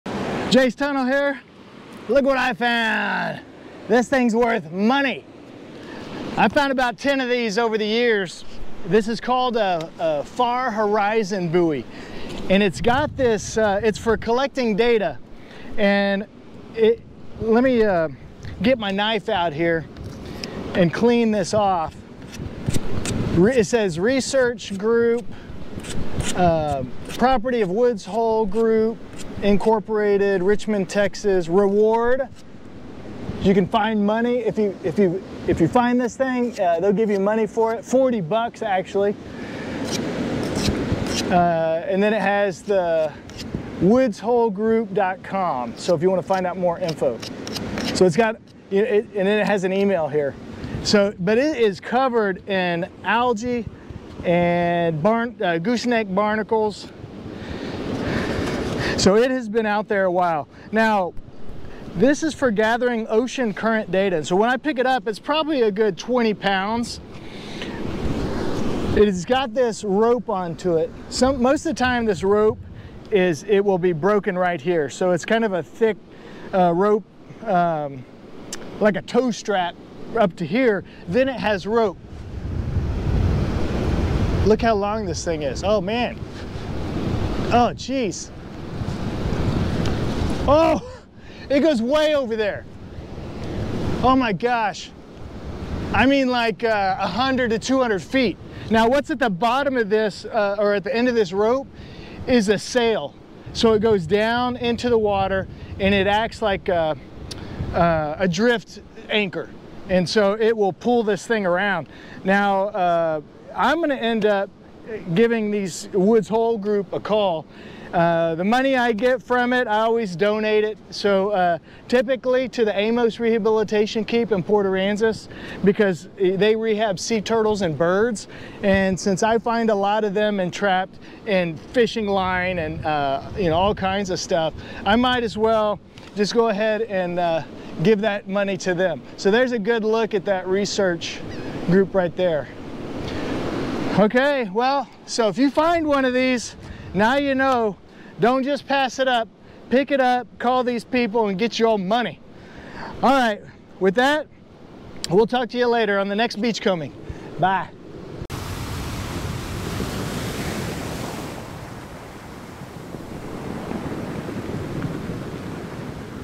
Beachcombing - Far Horizon Buoy
While beachcombing, I stumbled across something way more interesting than shells — a Far Horizon Buoy that had washed ashore. 🌊🔎

These buoys are scientific instruments designed to drift with the ocean, collecting valuable data on ocean currents that scientists use to better understand how our seas move and change over time.

The best part? Returning the buoy earns a $40 reward, but the real value is knowing the data it collected helps researchers track ocean circulation and improve marine science. In this video, I break down what the buoy is, how it works, and why finding one on the beach is such a cool (and important) discovery.